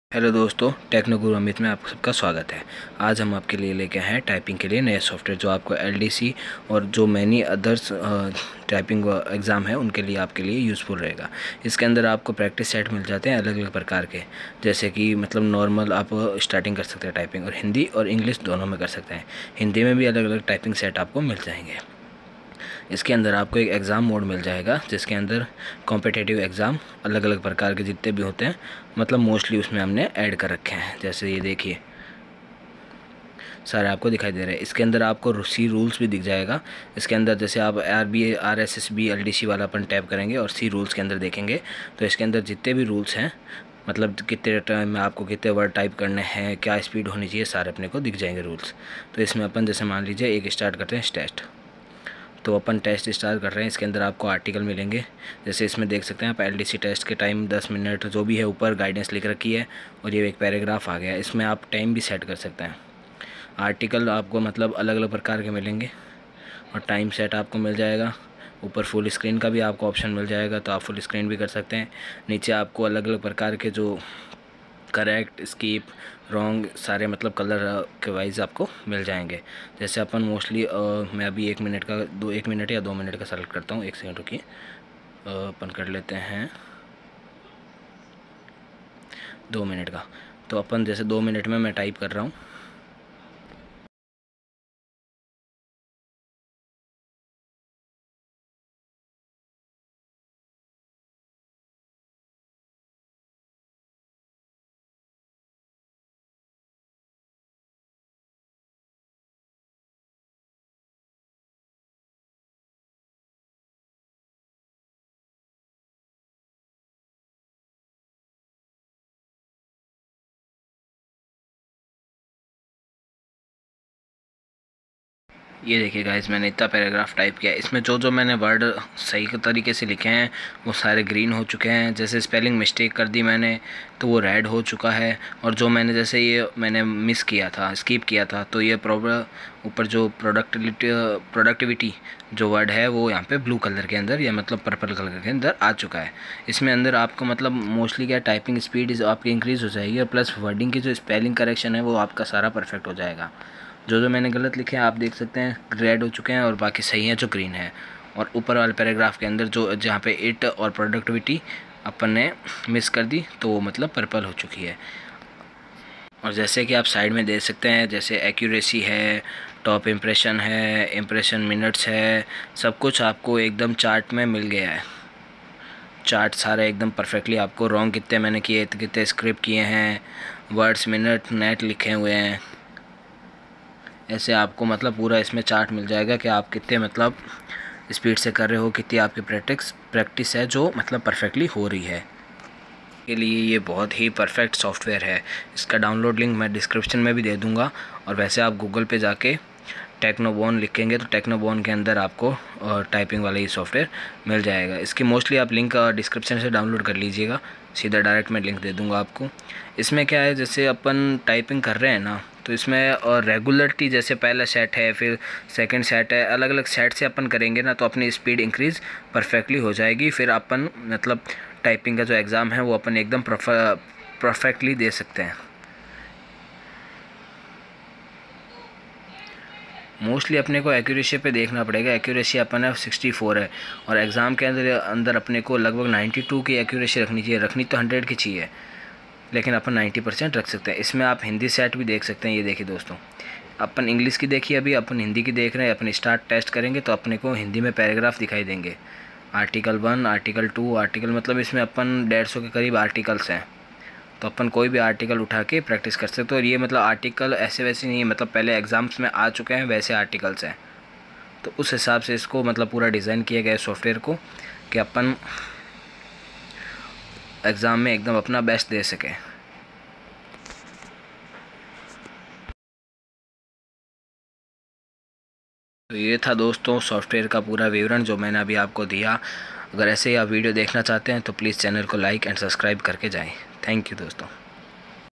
Best Typing Software For LDC // SSC // CHSL result out
If you want to know more about Hindi and English Typing then you can check my Typing lessons..

Download the “techno typing Pro software”. Perfect software to learn Hindi and English Typing at home.

Tech Tutorials and Computer Classes..